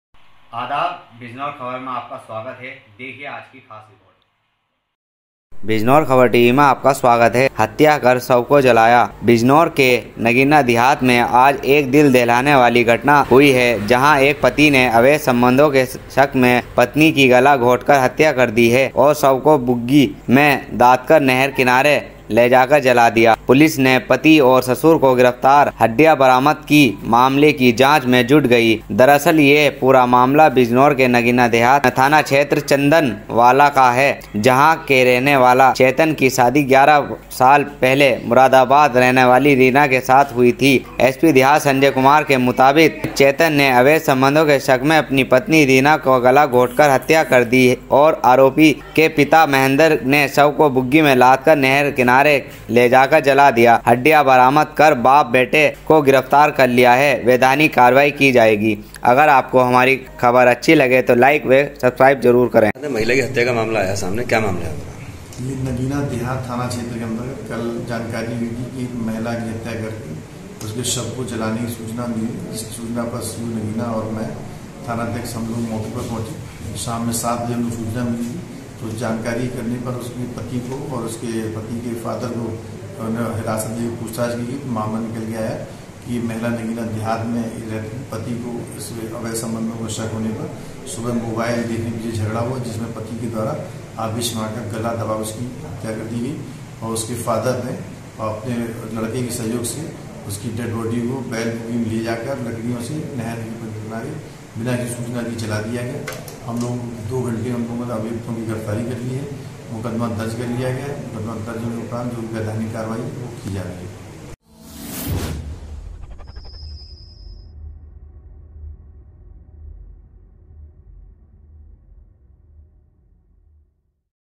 BIJNOR KHABAR || नगीना पति ने अवैध संबंधों के शक में पत्नी की गला घोटकर हत्या शव को जलाया
स्लग:- हत्या कर शव जलाया
रिपोर्ट:- फैसल खान  बिजनौर
एंकर:-बिजनौर के नगीना देहात में आज एक दिल दहला देने वाली घटना हुई जंहा एक पति ने अवैध सम्बन्धो के शक में पत्नी की गला घोंट कर हत्या कर दी और शव को बुग्गी में लादकर नहर किनारे लेजाकर जला दिया। पुलिस ने पति और ससुर को गिरफ्तार हड्डिया बरामद की मामले की जांच में जुट गई है।
दरअसल यह पूरा मामला बिजनौर के नगीना देहात थाना क्षेत्र  के चन्दन वाला का जंहा के रहने वाले चेतन की शादी 11 साल पहले मुरादाबाद की रहने वाली  रीना के साथ हुई थी। एसपी देहात संजय कुमार के मुताबिक चेतन ने अवैध सम्बन्धों के शक में अपनी पत्नी रीना की गला घोंट कर हत्या कर दी और आरोपी के पिता महेंद्र ने शव को बुग्गी पर लादकर नहर किनारे के लेजाकर जला दिया हड्डिया बरामद कर बाप बेटे को गिरफ्तार कर लिया वैधानिक कार्यवाही की जा रही है।
बाइट :-संजय कुमार एसपी ग्रामीण